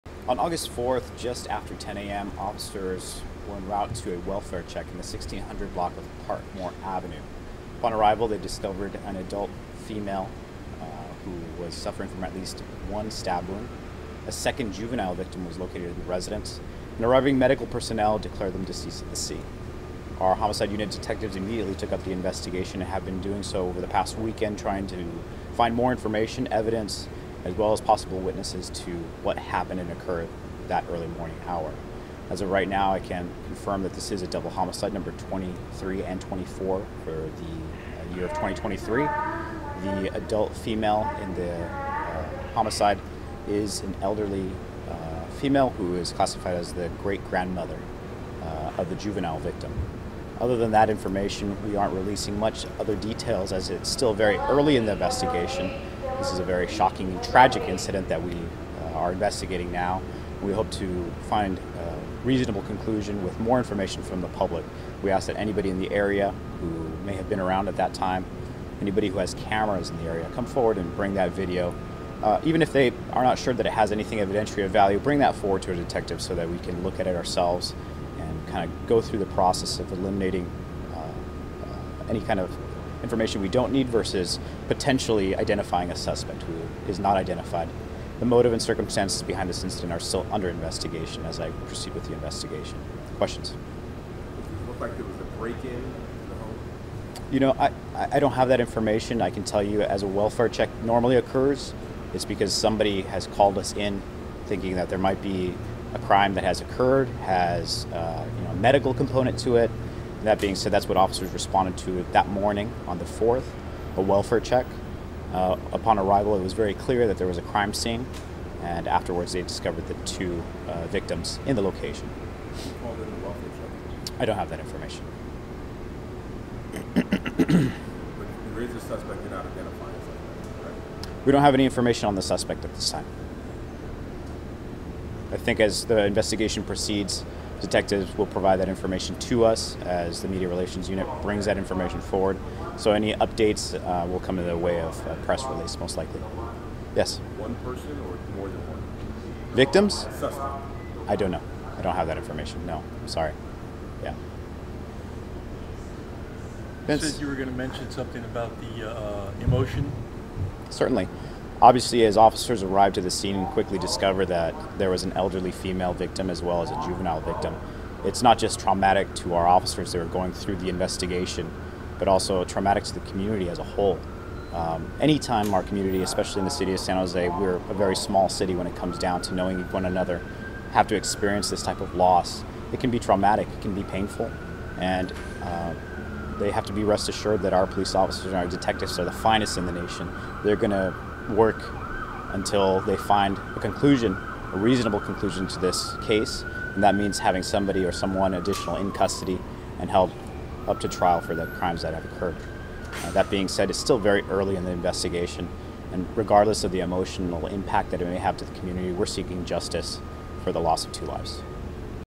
Elderly woman and child stabbed to death in San Jose
San Jose police officers found a double homicide scene that was emotionally traumatizing to see first-hand. "There was an elderly female victim, as well as a juvenile victim," suffering from fatal stab wounds when officers arrived at the home, Sgt. Steve Aponte said. "This type of loss can be traumatic. We are seeking justice," Aponte said. Police declined to tell reporters if a suspect has been identified or apprehended.